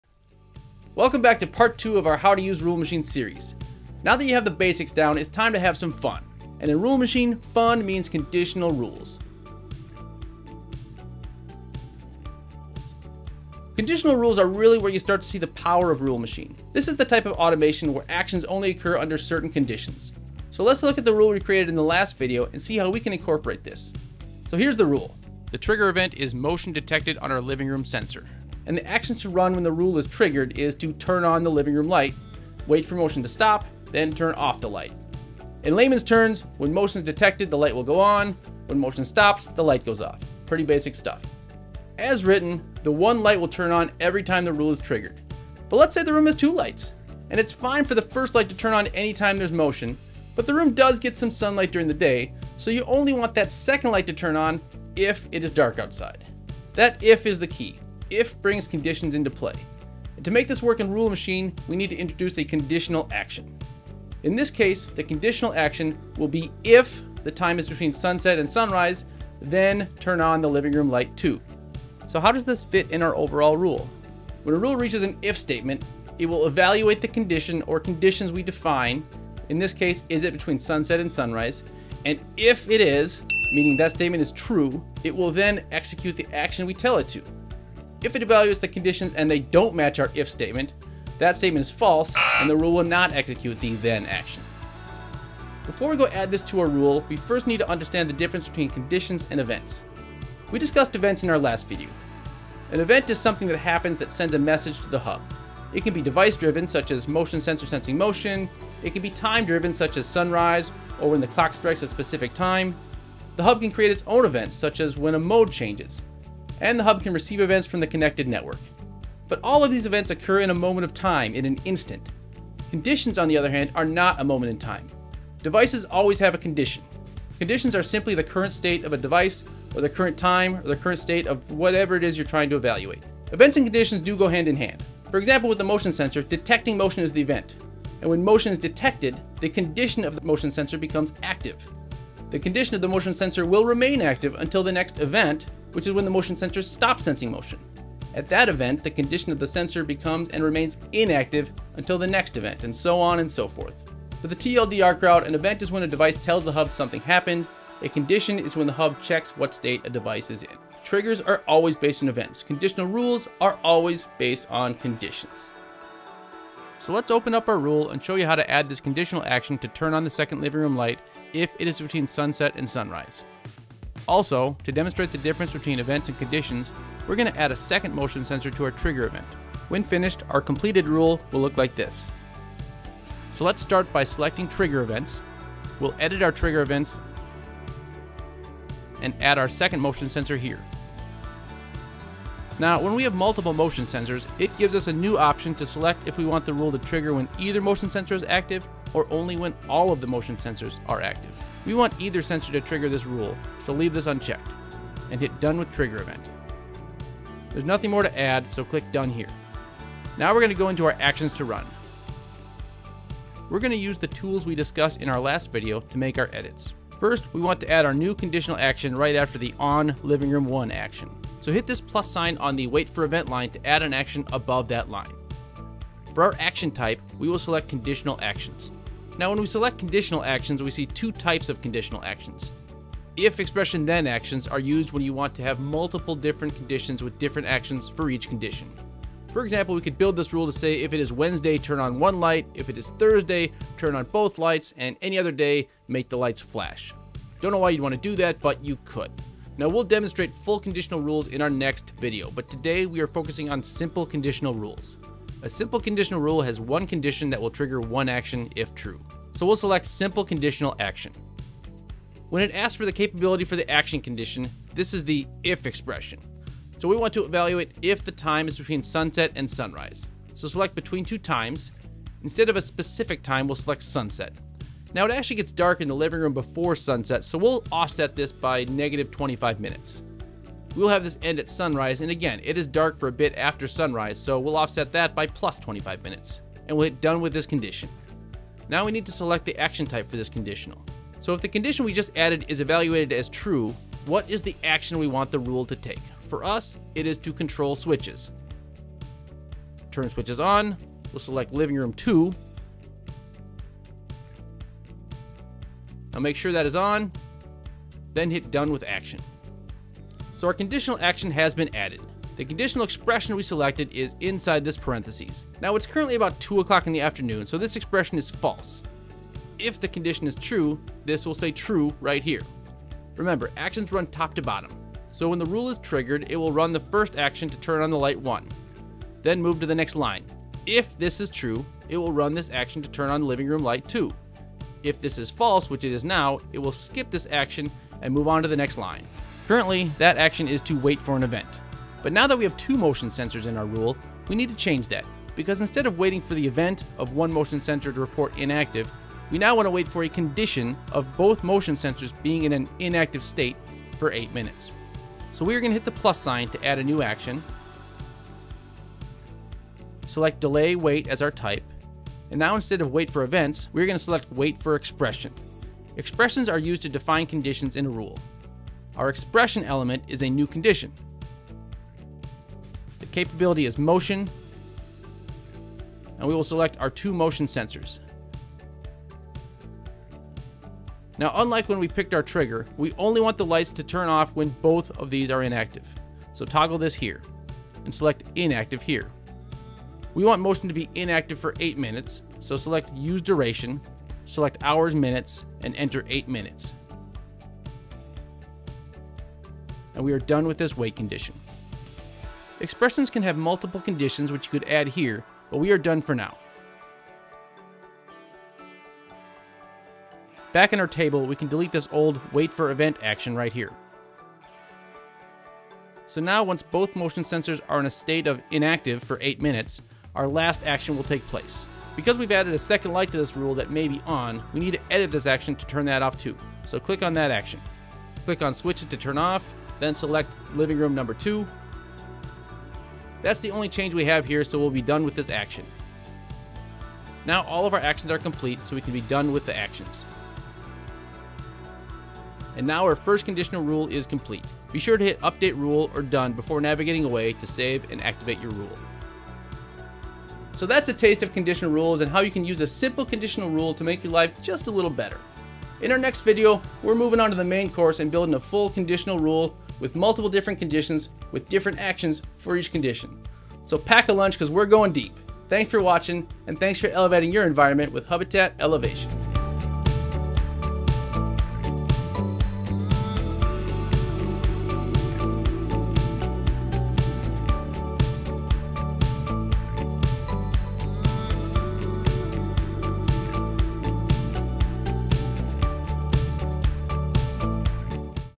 How to Use Rule Machine 2: Simple Conditional Rules
In Part 2 you'll learn what a conditional rule is and how to add a Simple Conditional Rule to your Rule Machine automations.

Rule Machine is the most advanced automation app in the Hubitat Elevation home automation hub platform.